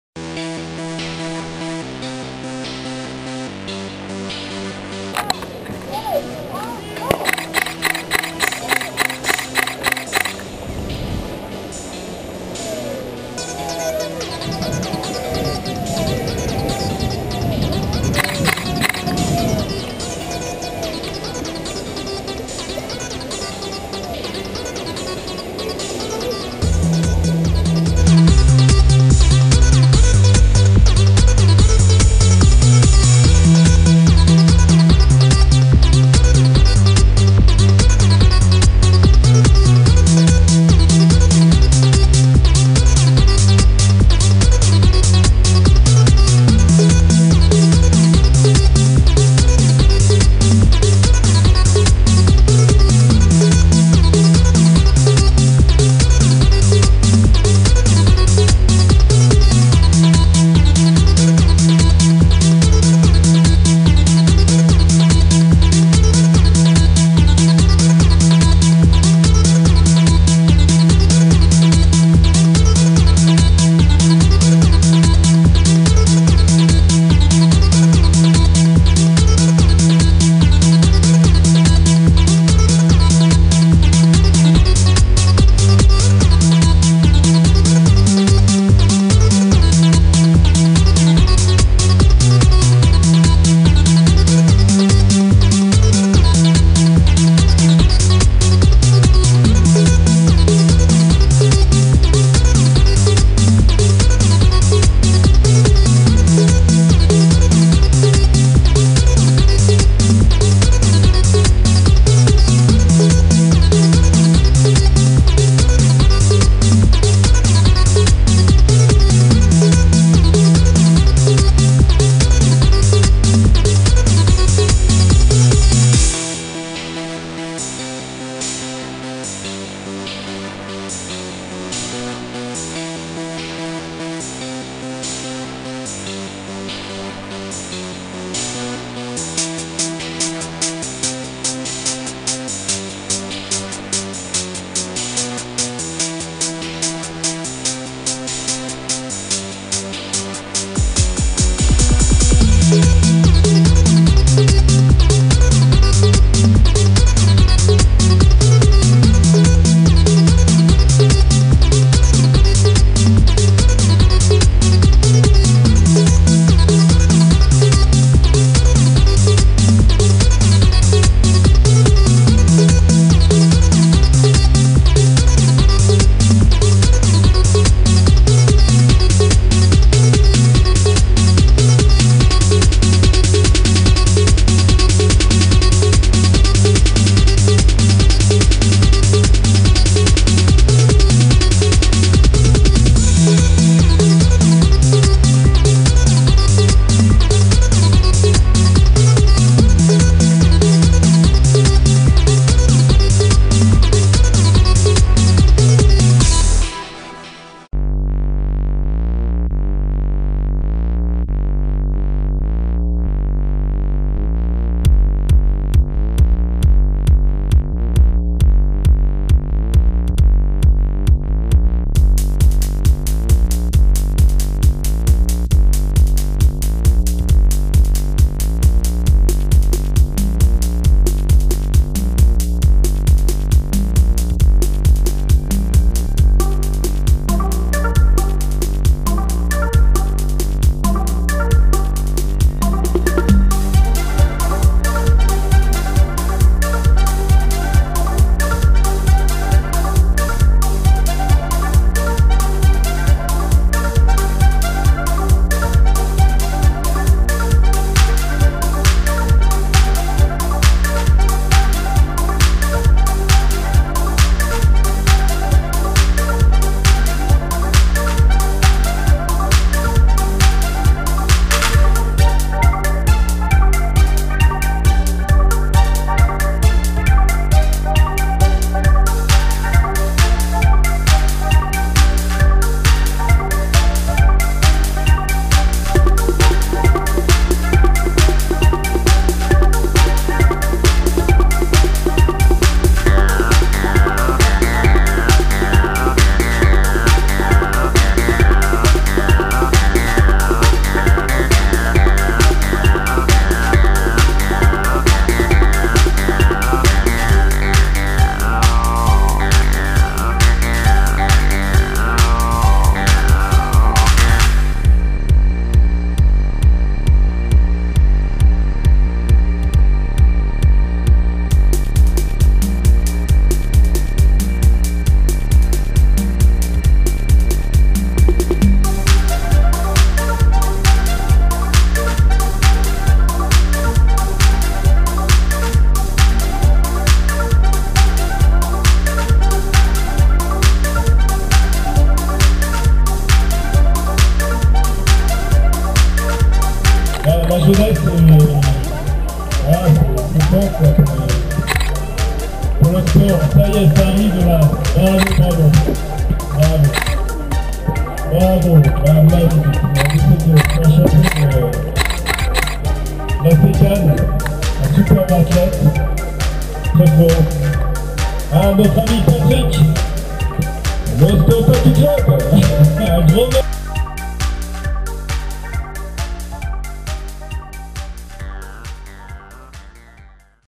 1er Aquathlon Cannes 24 septembre 2017
1er Aquathlon pour tous 24 septembre 2017
Boulevard du Midi
400m Natation + 1000m CPA une boucle ou 3 boucles
Tristars Cannes Triathlon